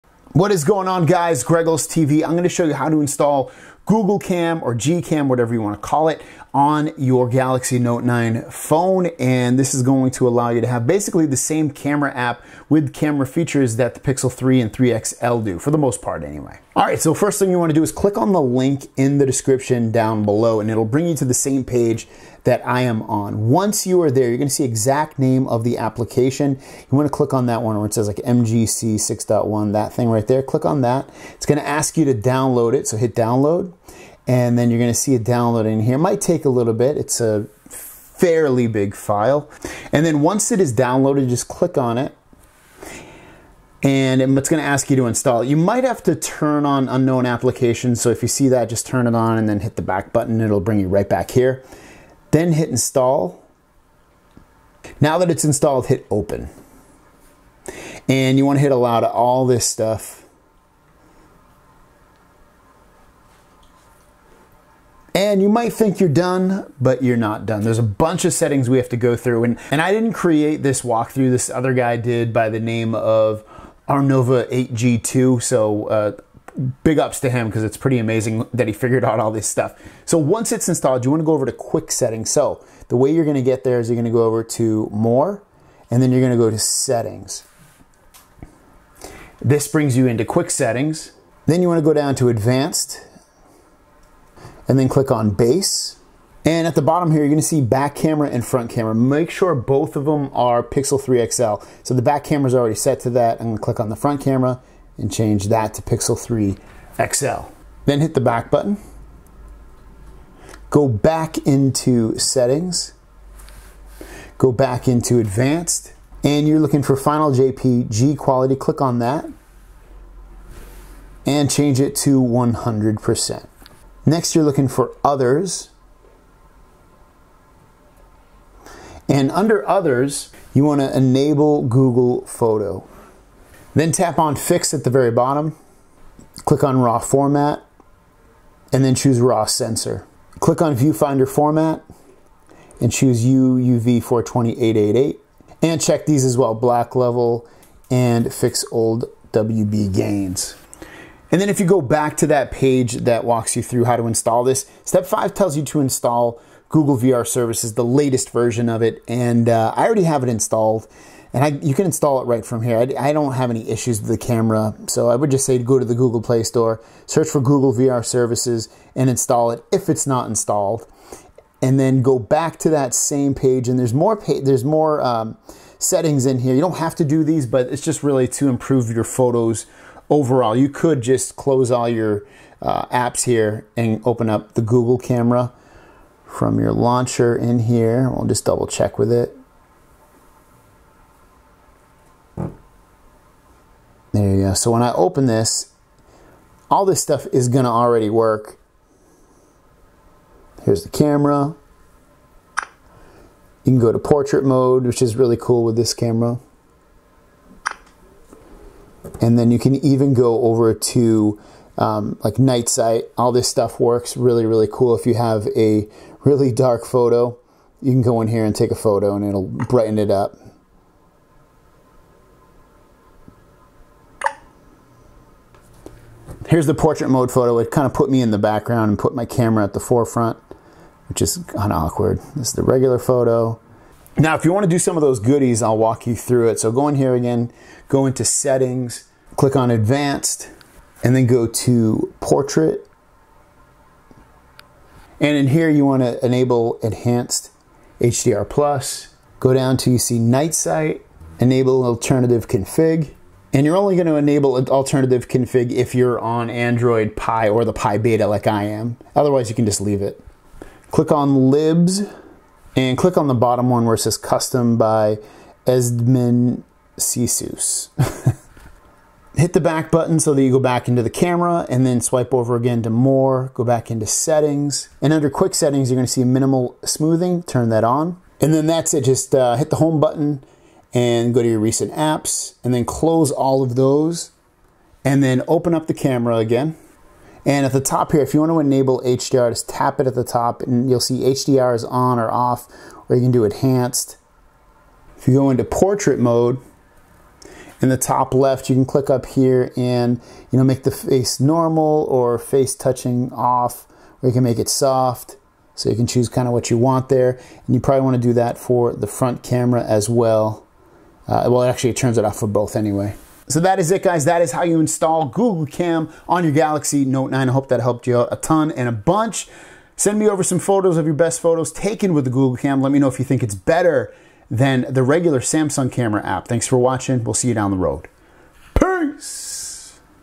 Galaxy Note 9 How to Install GCam Walkthrough
Galaxy Note 9 How to Install Google Pixel 3 Camera Walkthrough

NEW GregglesTV T-Shirts & Hoodies